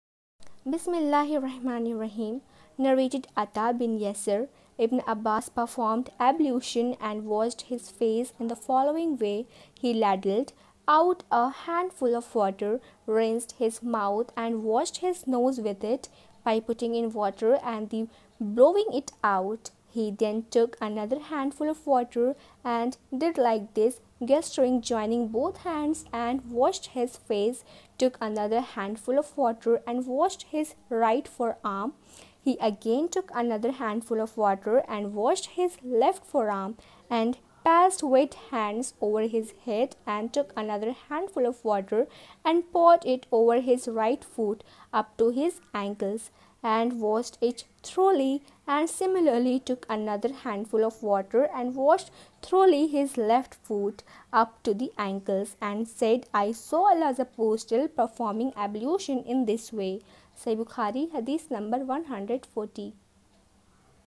Sahih Bukhari Hadees English 140
Narrated `Ata' bin Yasar: Ibn `Abbas performed ablution and washed his face (in the following way): He ladled out a handful of water, rinsed his mouth and washed his nose with it by putting in water and then blowing it out. He then, took another handful (of water) and did like this (gesturing) joining both hands, and washed his face, took another handful of water and washed his right forearm. He again took another handful of water and washed his left forearm, and passed wet hands over his head and took another handful of water and poured it over his right foot (up to his ankles) and washed it thoroughly and similarly took another handful of water and washed thoroughly his left foot (up to the ankles) and said,   I saw Allah's Apostle performing ablution in this way.